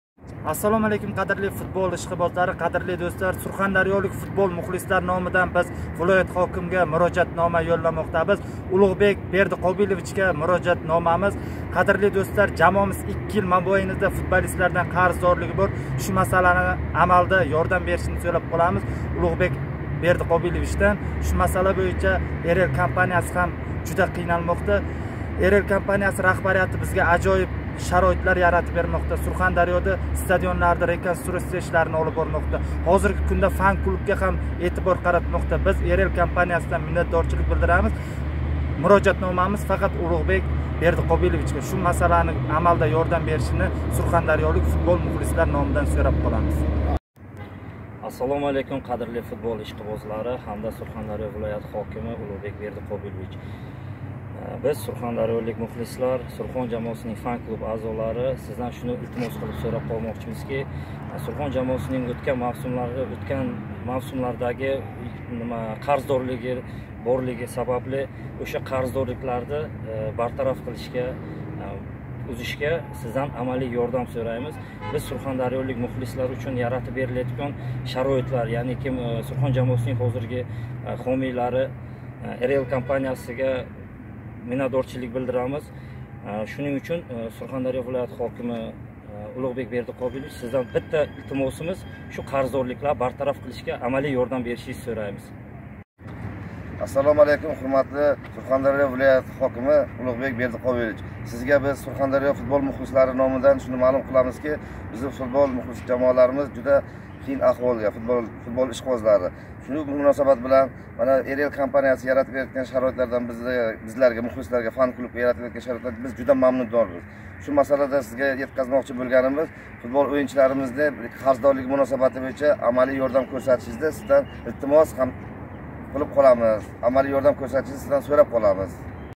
Мурожаат Сурхондарё вилояти ҳокими Қосимов Улуғбек Бердиқобиловичга
Сурхондарё вилояти ҳокими Қосимов Улуғбек Бердиқобиловичга Сурхондарёлик футбол мухлислари номидан видео мурожаат!